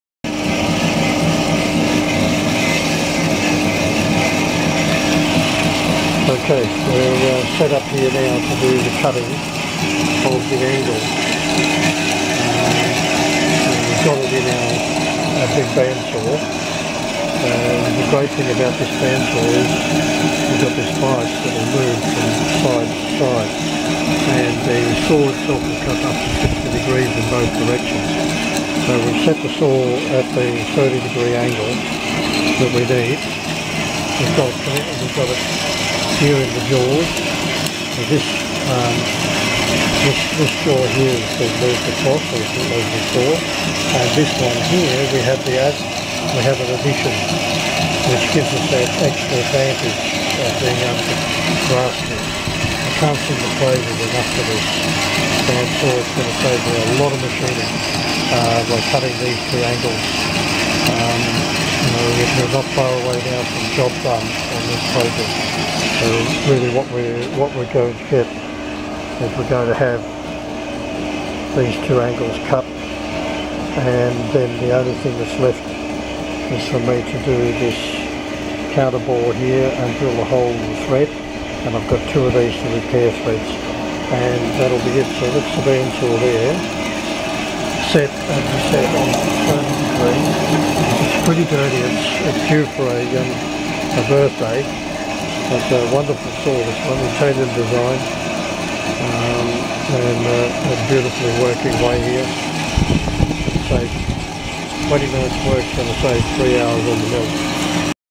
Broken Jaw Saga Bandsaw
Broken Jaw Saga Band-sawing the two cheeks of the jaw holder to save machining time. We show the multi set horizontal bandsaw in action, 30 degree saw offset, vice shifted on the Y axis and vice jaw extender plate. all working to reduce machining time.